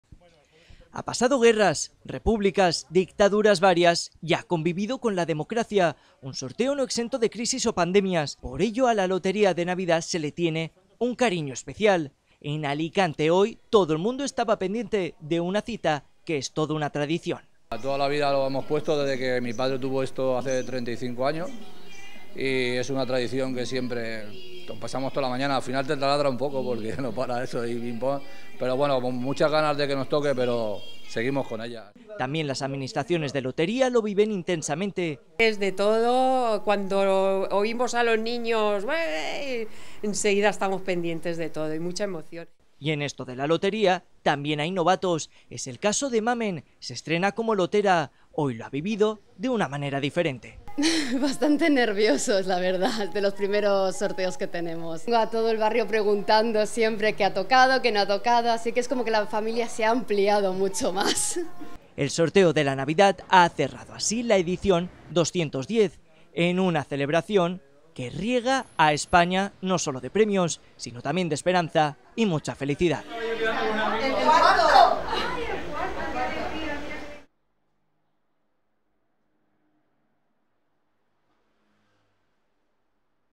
Así se ha vivido el sorteo de la Lotería de Navidad 2022 en Alicante
Sintonízanos
en el canal 21 de la TDT